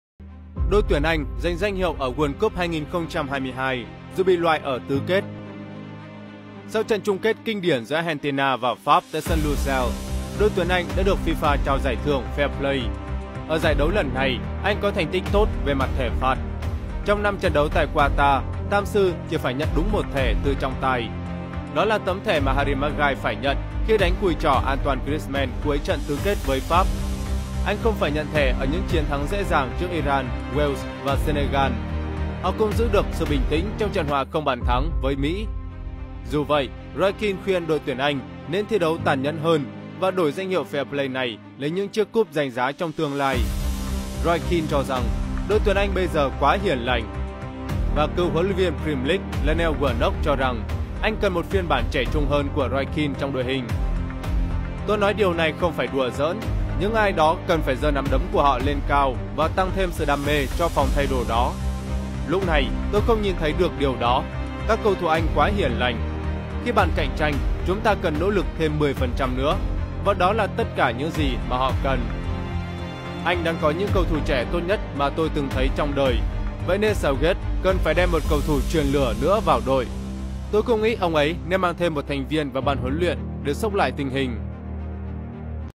Tin Thể Thao Bóng Đá: ĐT Anh giành danh hiệu ở World Cup 2022
ĐT Anh giành danh hiệu ở World Cup 2022 dù bị loại ở tứ kết